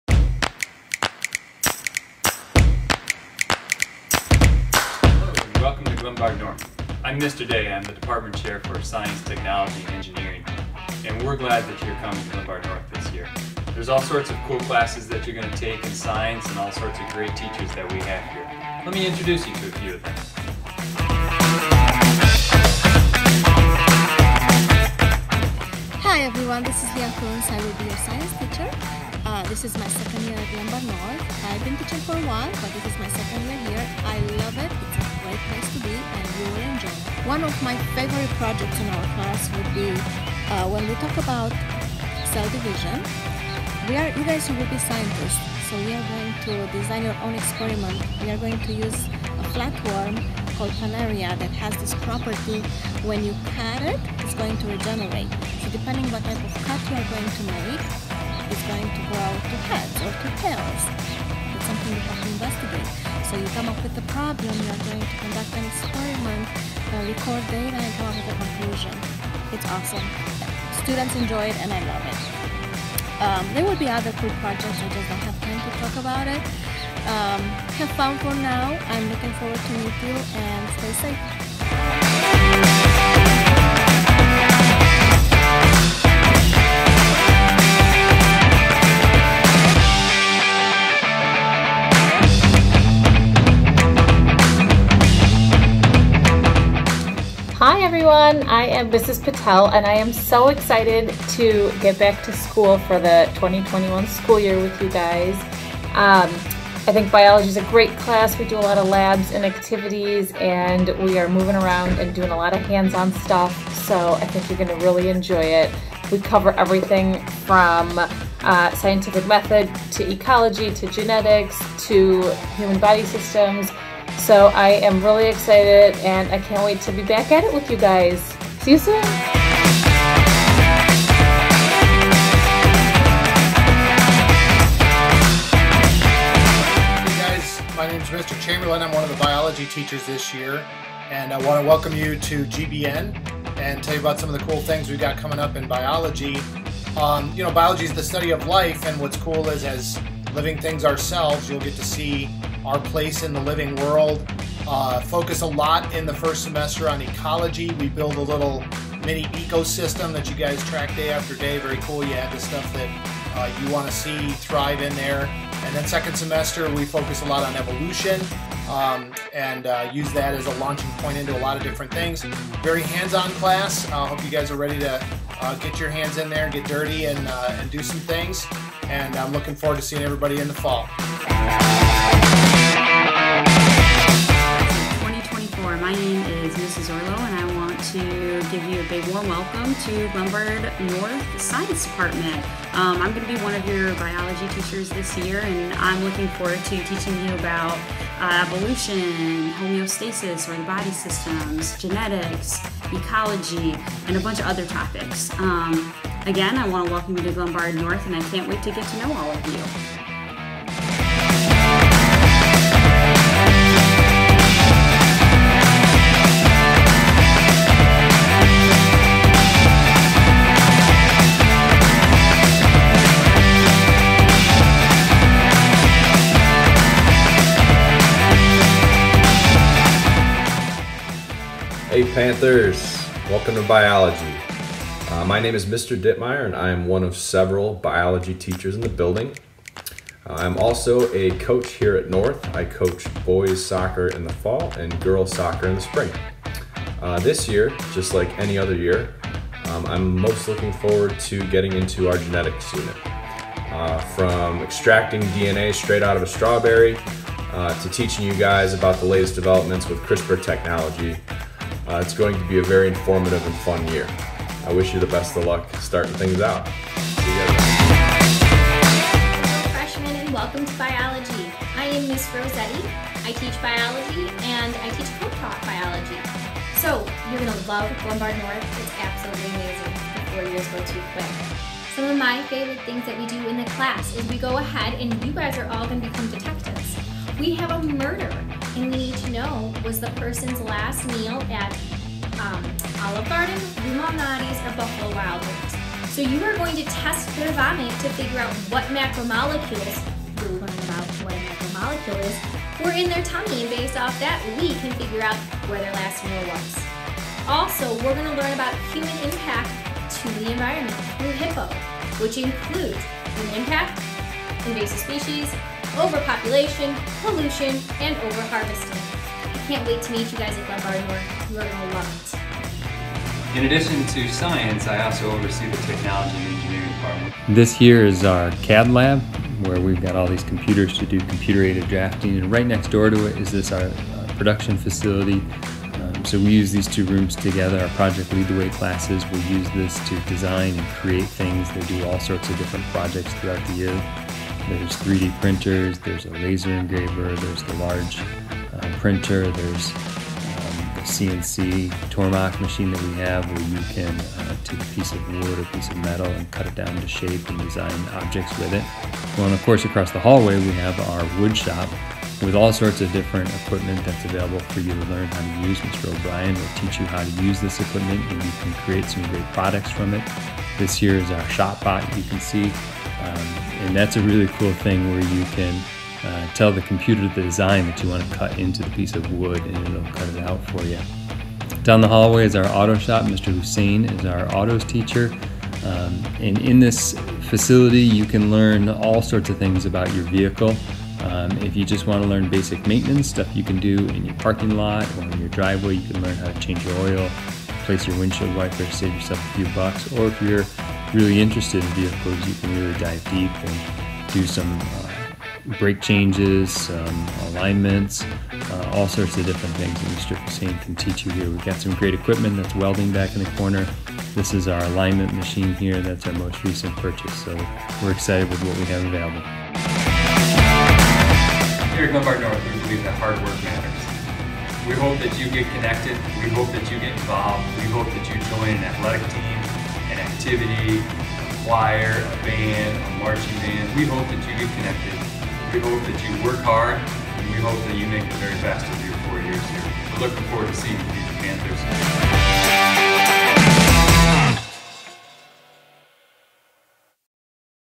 Welcome to Biology at Glenbard North
A welcome video from the Science Department at Glenbard North for our incoming Freshmen.